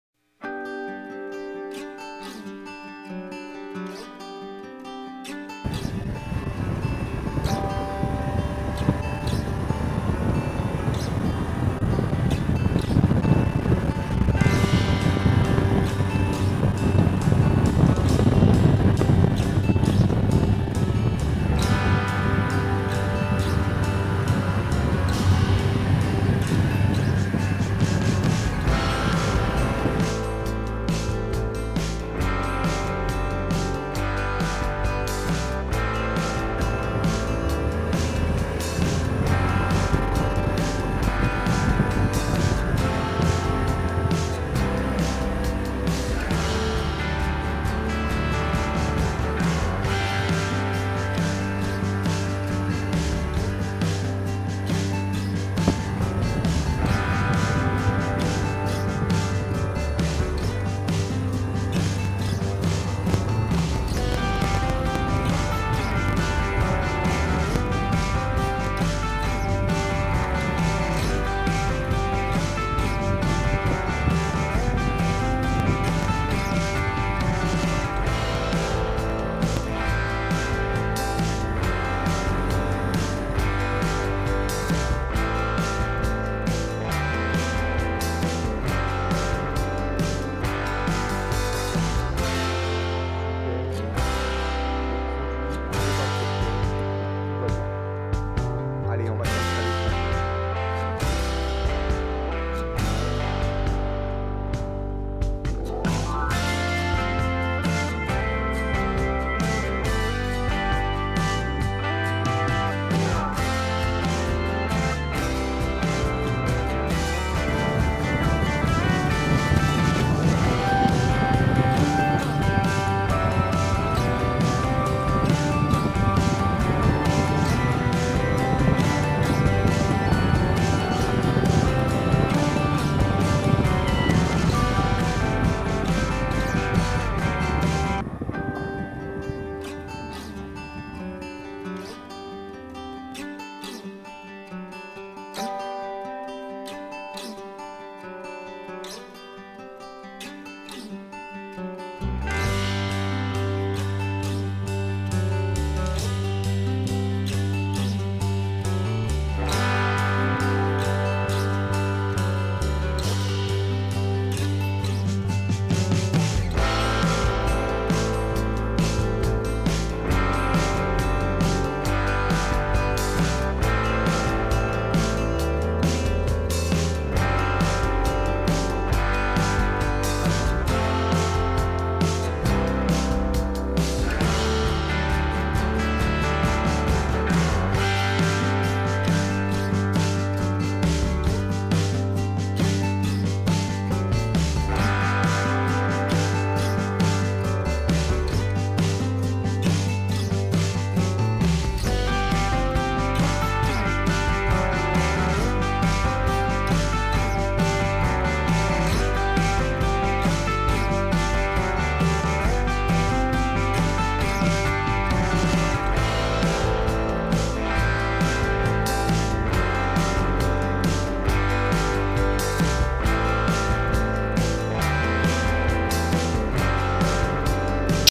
Trip moto avec bivouac au col de st jean - 01/05/2022
Trip moto dans les Baronnies Provençales au départ de Gap en passant le long de la Durance,  les gorges de la Méouge, le col St jean avec bivouac, le village de Orpierre et retour à Gap en par le village de Savournon.